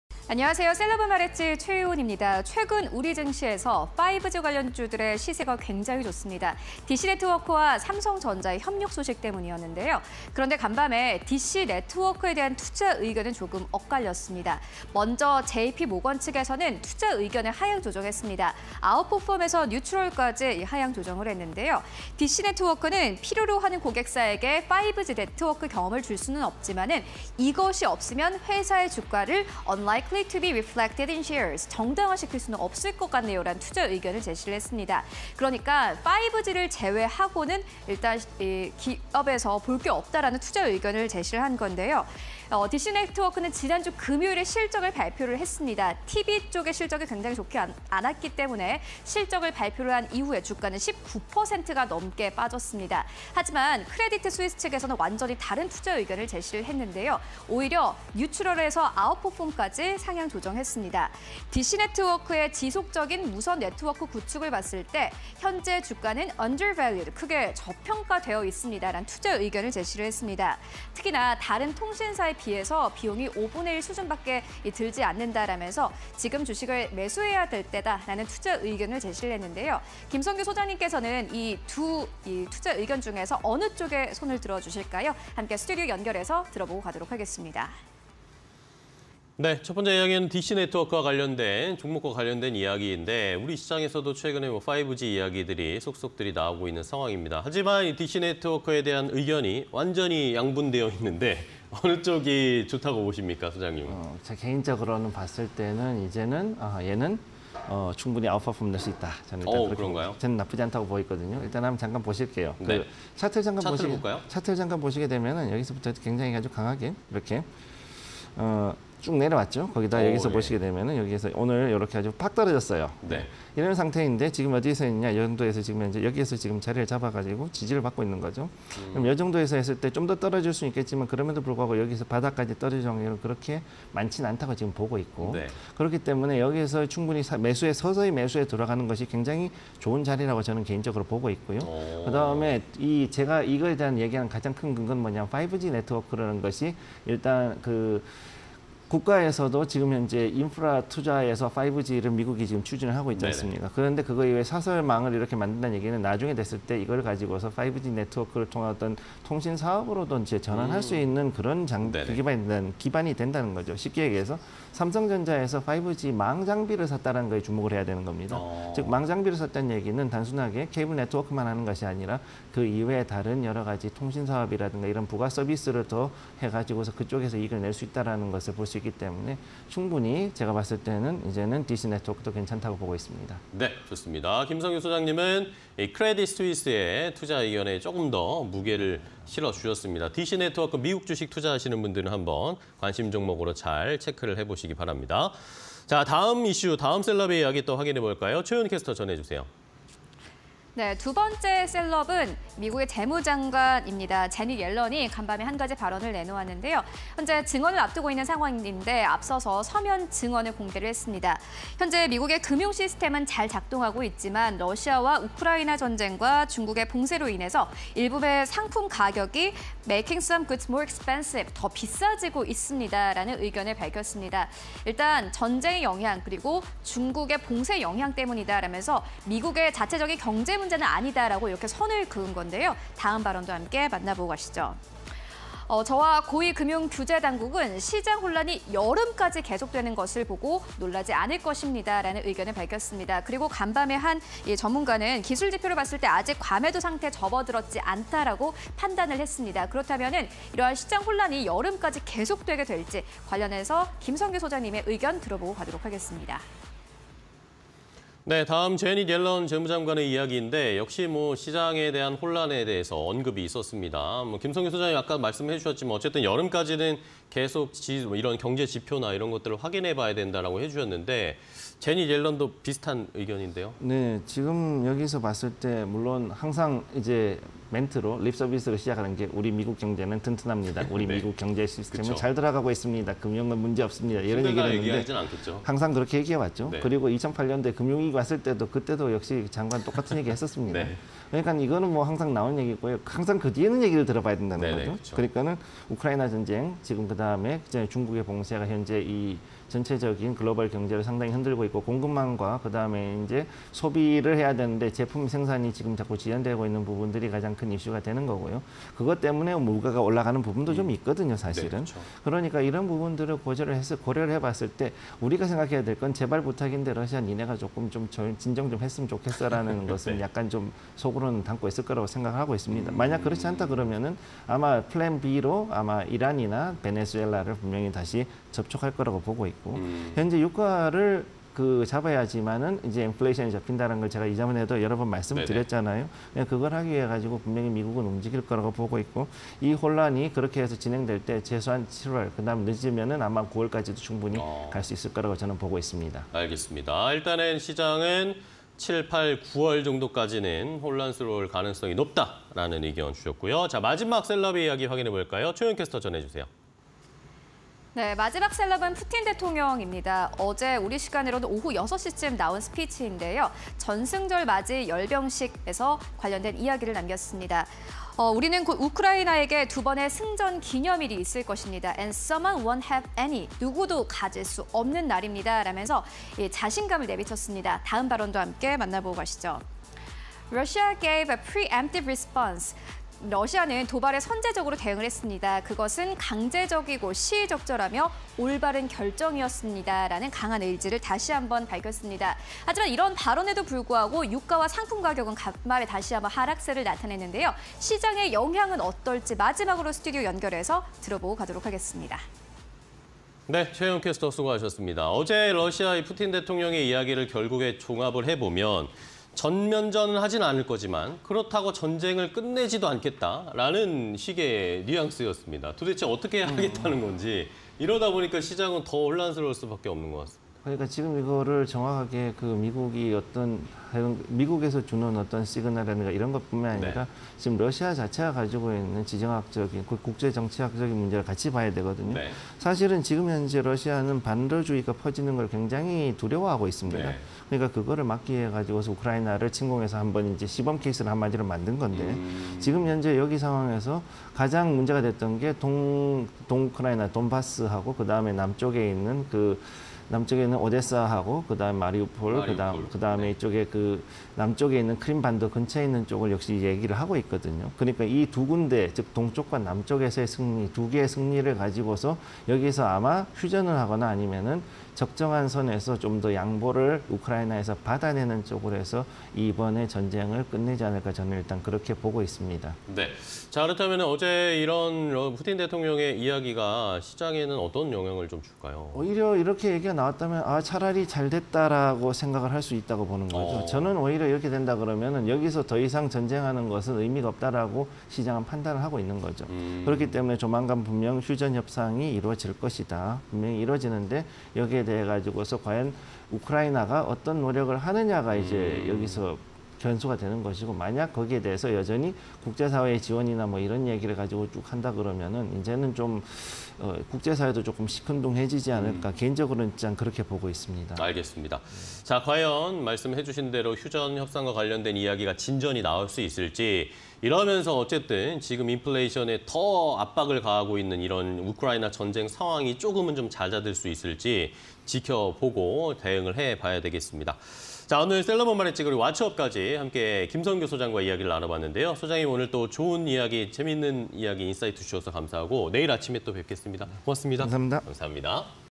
필립 쿠식(JP모건 애널리스트) / 더글라스 미첼슨(크레디트 스위스 애널리스트) / 제닛 옐런(미국 재무장관) / 블라디미르 푸틴(러시아 대통령)_셀럽은 말했지 (20220510)
필립_쿠식(JP모건 애널리스트) / 투자서한
The company can "create a differentiated 5G network experience that enterprises need to use, without which the company’s massive spectrum holdings seem unlikely to be reflected in shares."
디시네트워크는 필요로 하는 고객사에게 5G 네트워크 경험을 줄 수는 있지만, 이것이 없으면 회사의 주가를 정당화시킬 수는 없을 것 같네요.

더글라스_미첼슨(크레디트 스위스 애널리스트) / 투자서한
"The company’s ongoing buildout of its wireless network was undervalued by the current stock price."
디시네트워크의 지속적인 무선 네트워크 구축을 봤을 때, 현재 주가는 크게 저평가되어 있습니다.

제닛_옐런(미국 재무장관) / 서면 증언
"I belive the U.S. financial system is in working order, but that Russia’s invasion of Ukraine and China’s Covid-19 lockdowns are making some goods more expensive."
미국의 금융 시스템은 잘 작동하고 있지만, 러시아-우크라 전쟁과 중국의 봉쇄로 인해 일부 상품 가격은 더 비싸지고 있다고 생각합니다.

"Me and other top financial regulators wouldn’t be surprised to see market turbulence keep up into the summer."
저와 고위 금융 규제 당국은 시장 혼란이 여름까지 계속되는 것을 보고 놀라지 않을 것입니다.

블라디미르_푸틴(러시아 대통령) / 전승절 맞이 열병식
"Very soon there will be two Victory Days in Ukraine. And someone won’t have any."
우리는 곧 우크라이나에게 두 번의 승전 기념일이 있을 것입니다. 누구도 가질 수 없는 날입니다.

"Russia gave a pre-emptive response to the aggression. It was a forced, timely and only correct decision."
러시아는 도발에 선제적으로 대응했습니다. 그것은 강제적이고 시의적절하며 올바른 결정이었습니다.